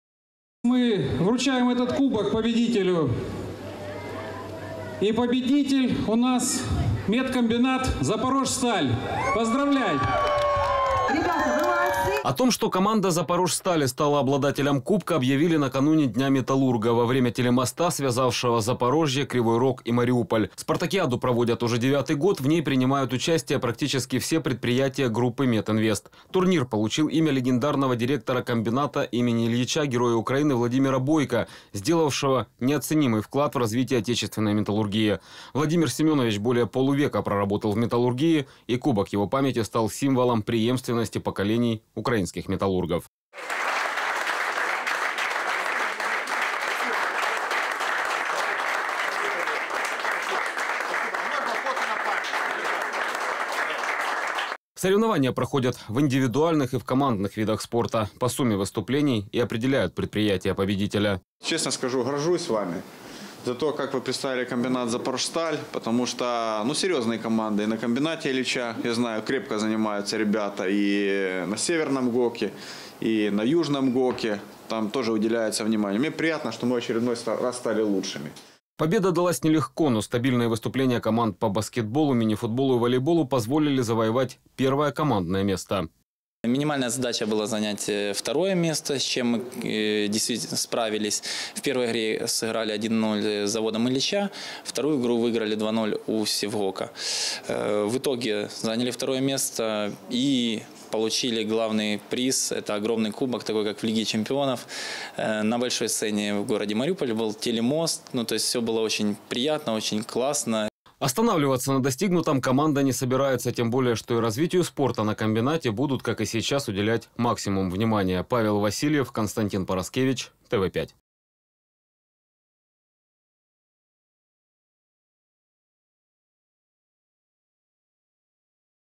Запорожские металлурги стали победителями спартакиады «Метинвеста»
Спортивные успехи: запорожские металлурги стали победителями спартакиады «Метинвеста» памяти Героя Украины Владимира Бойко. В командном зачете спортсмены комбината «Запорожсталь» заняли первое место и стали обладателем кубка, который получили к Дню металлурга в Мариуполе. Сегодня спортсменов с успехом поздравили руководители предприятия и пообещали — развитию спорта будут уделять максимум внимания.